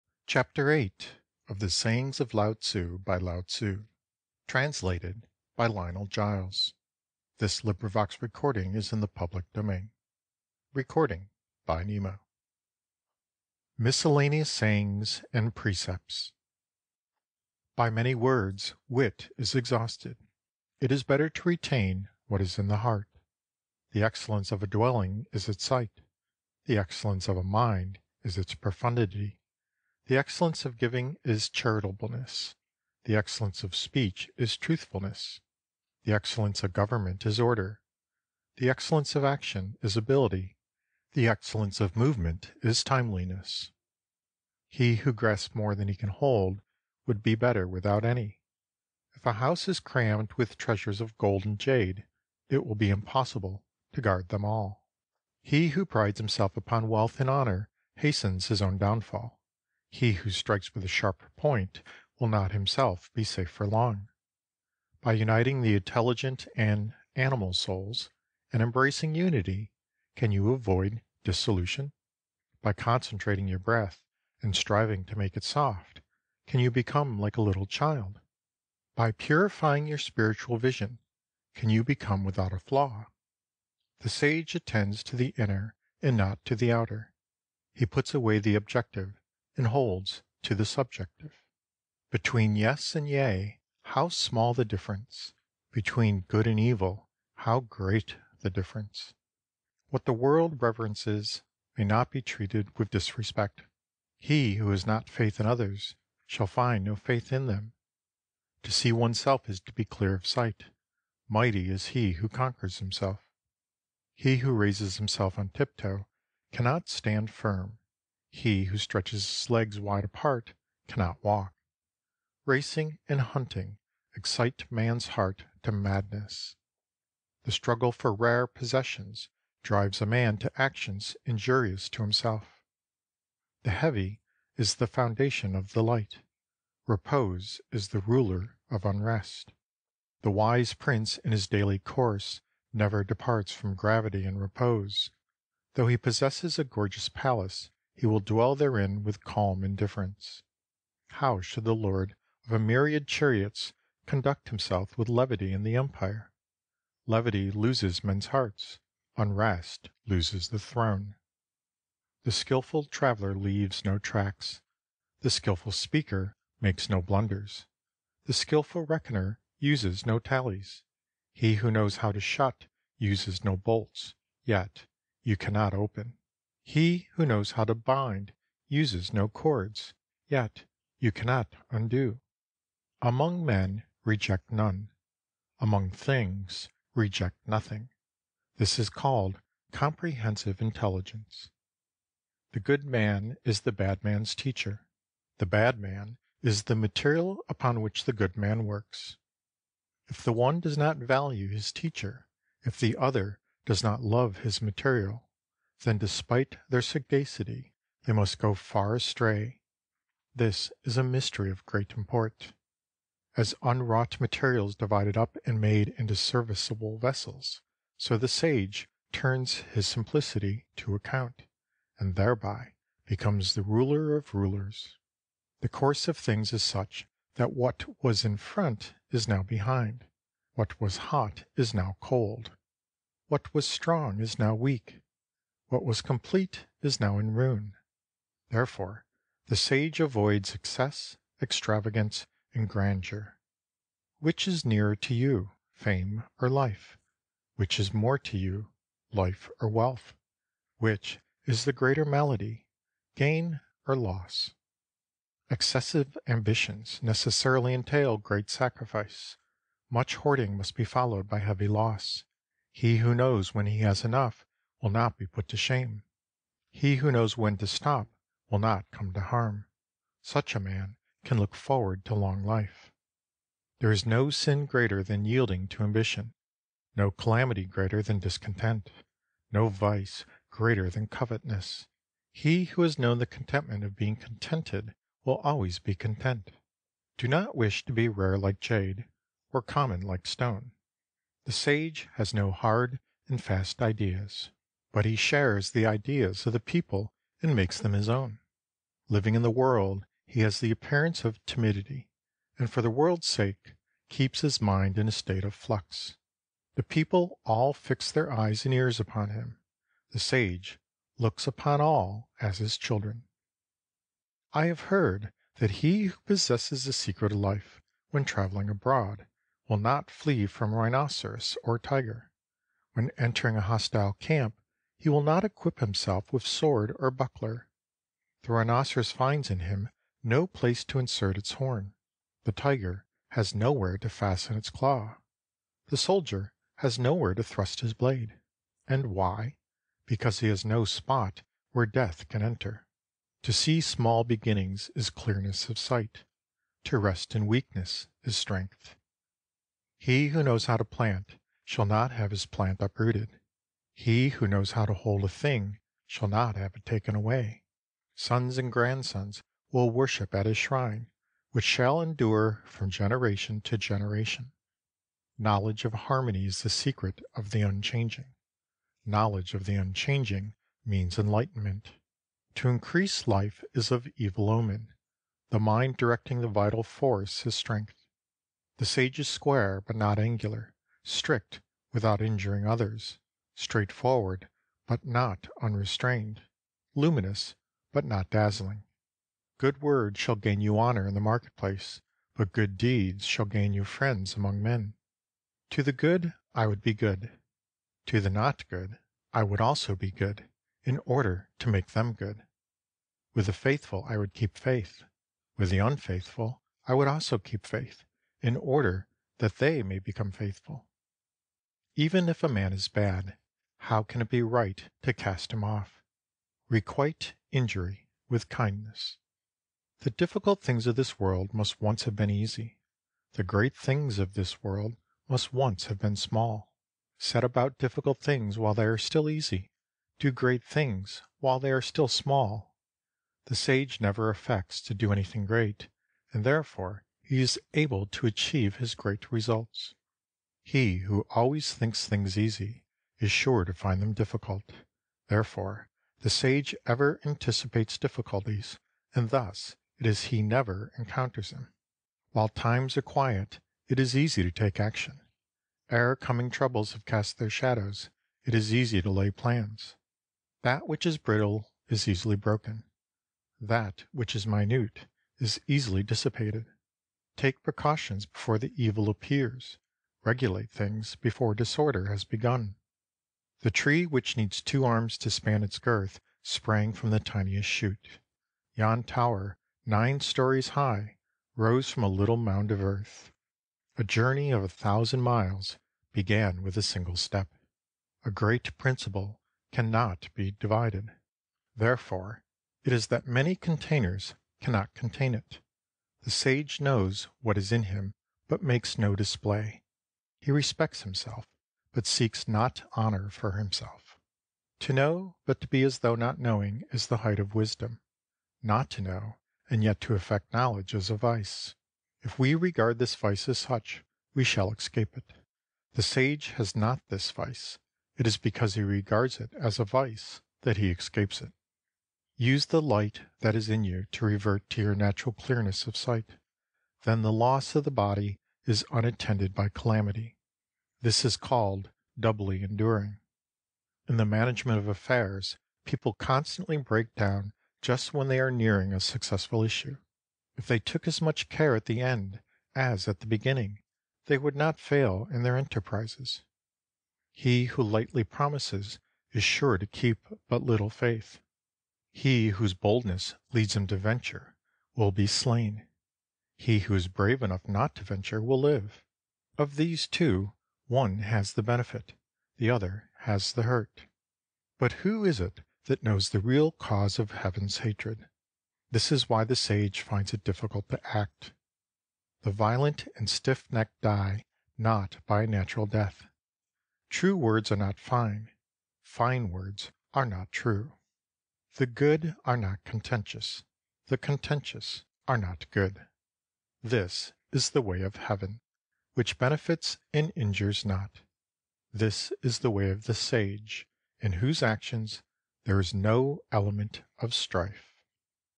Sayings and Precepts by Lao Tzu Ch. 8
The Sayings of Lao Tzu by Lao Tzu Chapter 8.  Sayings and Precepts

LibriVox recording of The Sayings of Lao Tzu by Lao Tzu. (Translated by Lionel Giles.)
Read in English by Nemo

Lao-Tzu, also known as Laozi was a Chinese philosopher believed to have lived in the 6th century BCE and is credited with writing the Tao-Te-Ching which centers around the idea that the way of virtue lies in simplicity and a recognition of a natural, universal force known as the Tao. He is traditionally regarded as the founder of Taoism. This book is a compilation of his most profound writings translated directly from ancient Chinese texts. - Summary by Nemo